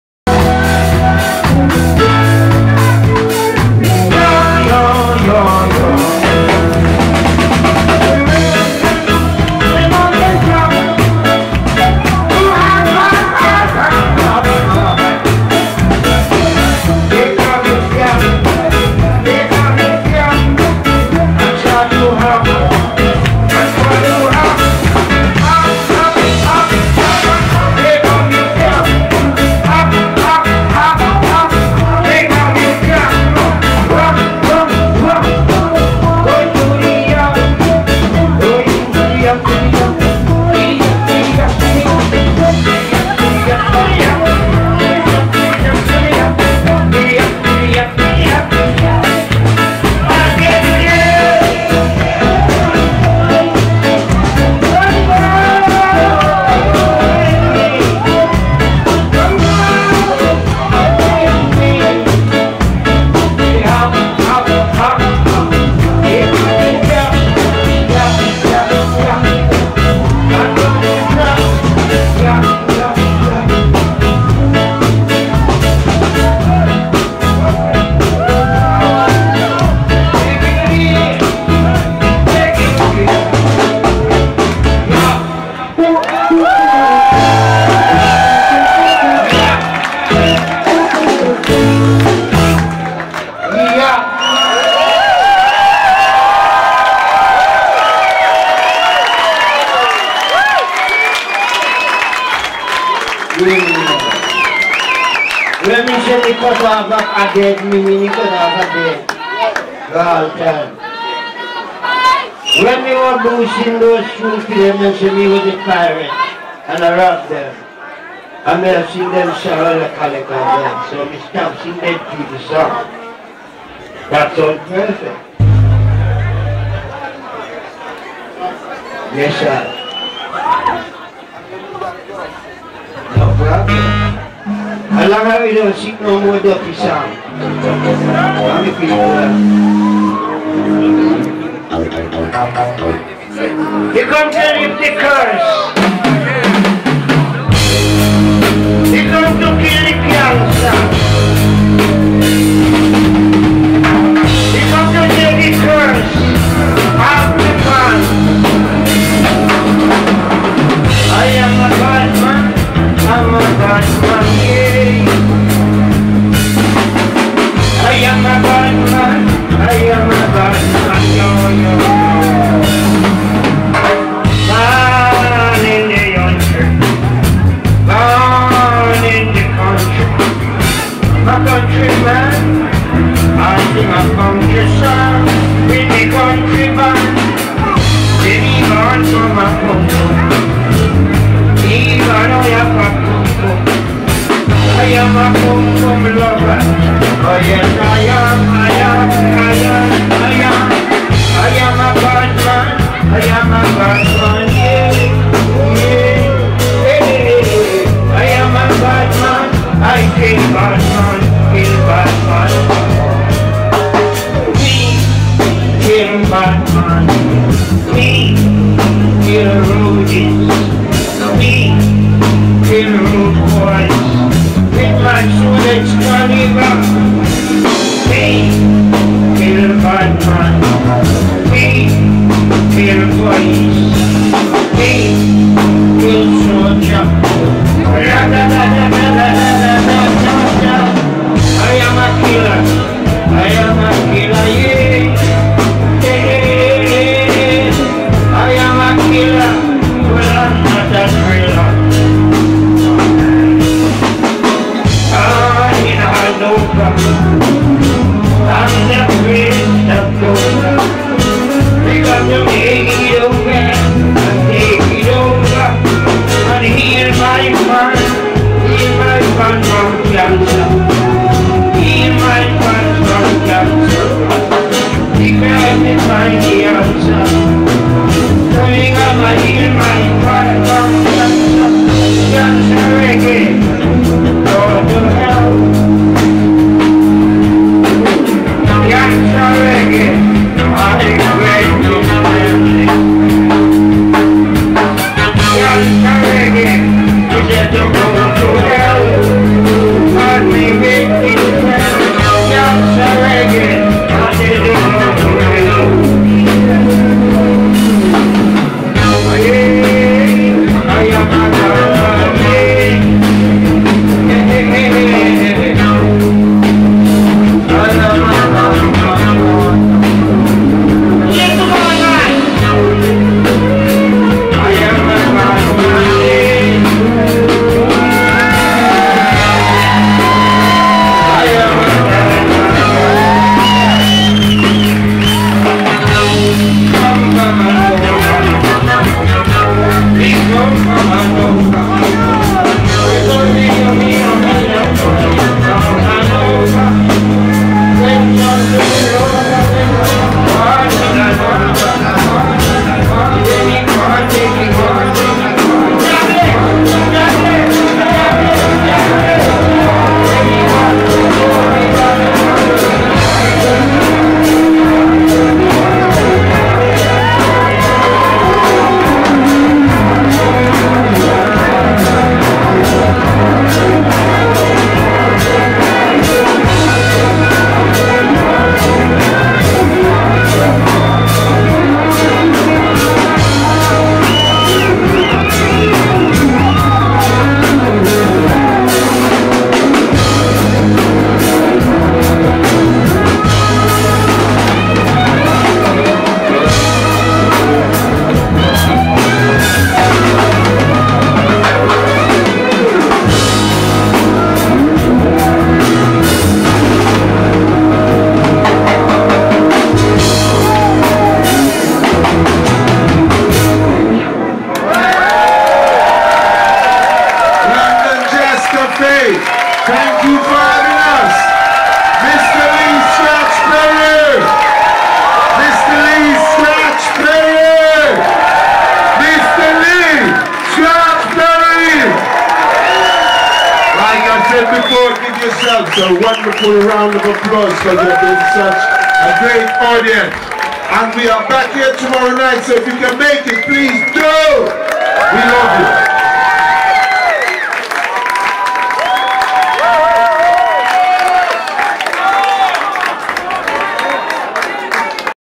Lee Scratch Perry with the Upsetters Band - Jazz Cafe, London pt.3
Scratch closing his first night at the Jazz Cafe, supported by the Upsetters Band. Great night of entertainment. JDL